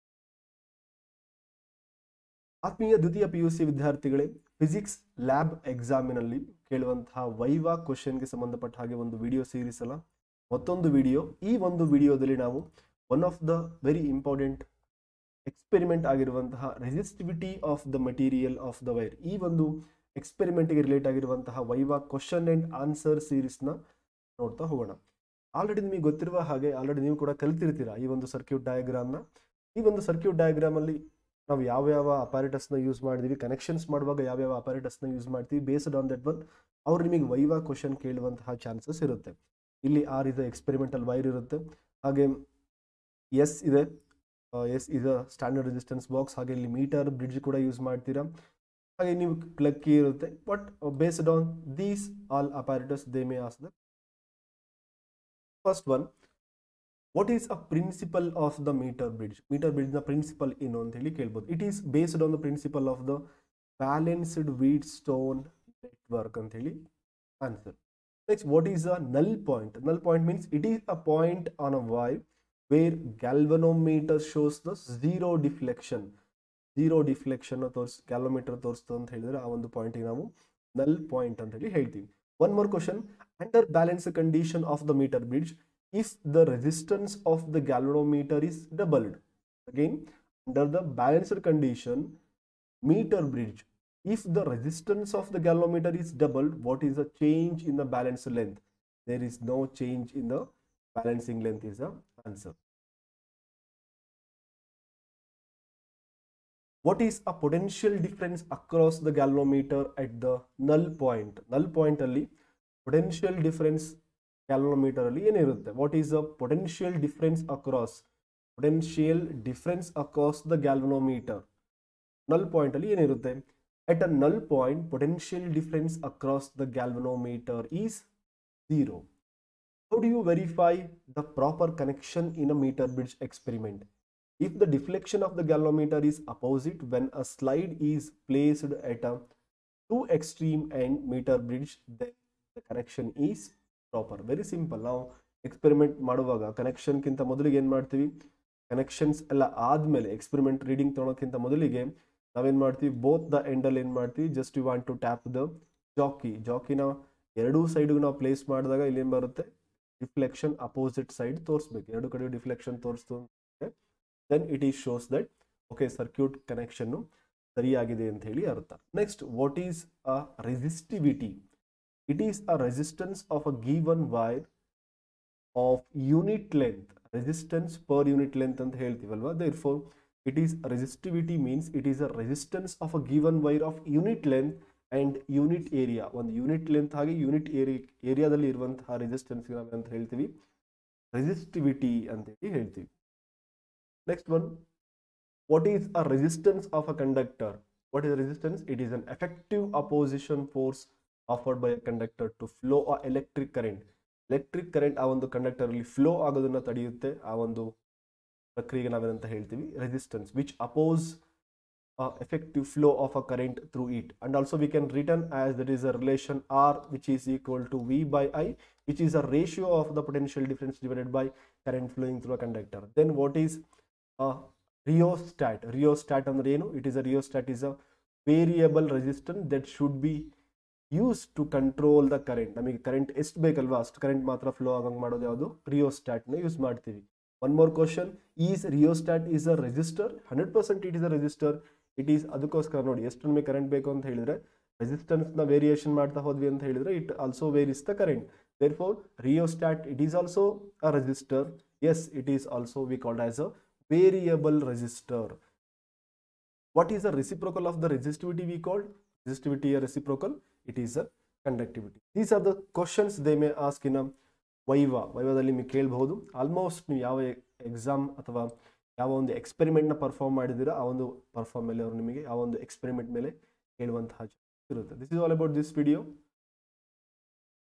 Commonly Asked Questions In 2puc Physics Practical (VIVA)| Resistivity of The Material of the Wire
KSET-2021: OFFICIAL KEY ANSWERS – KANNADA thelegend